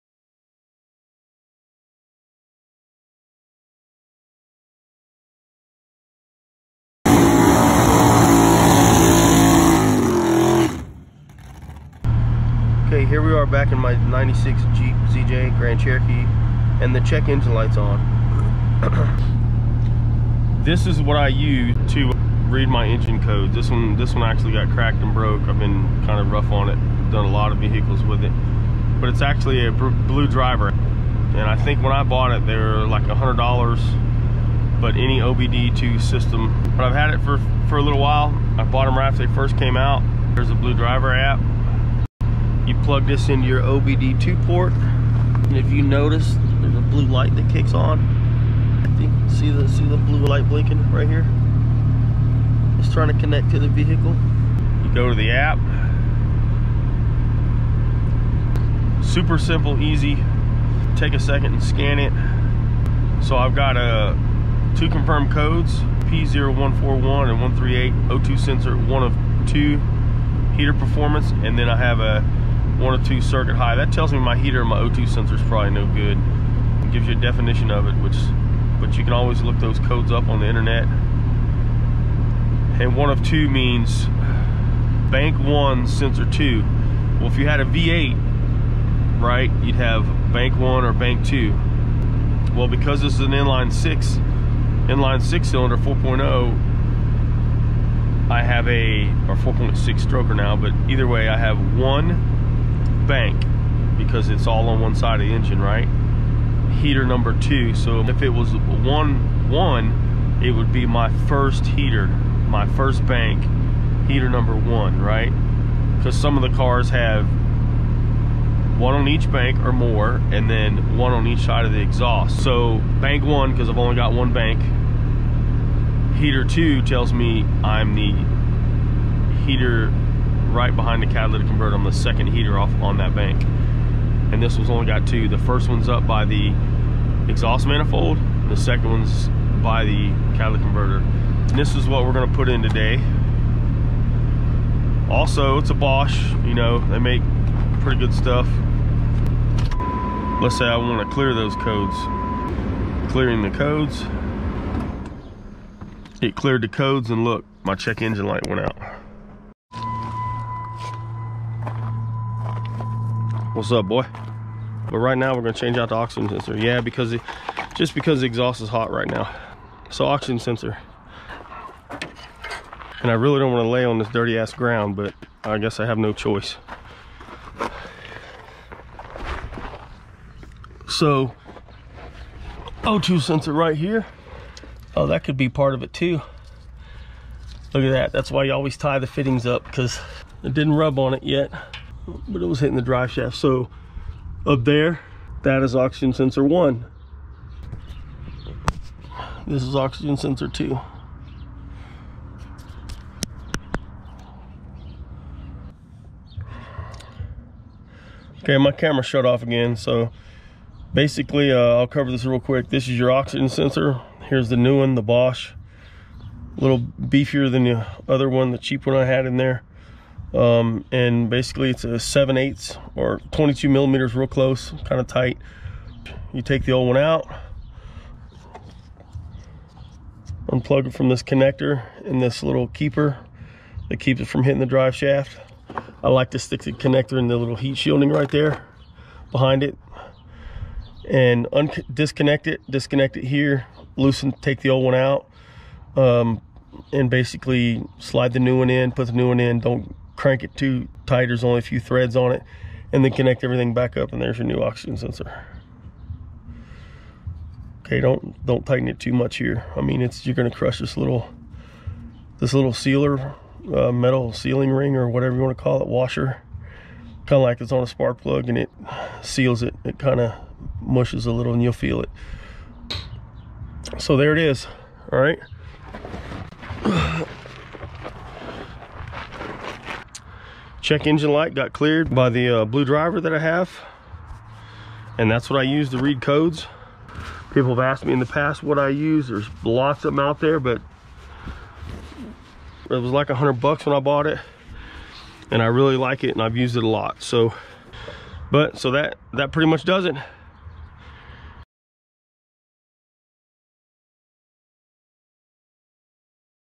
96 Jeep ZJ O2 sensor replacement
1996 Jeep ZJ Check engine light, codes P0141, P0138, and O2 sensor replacement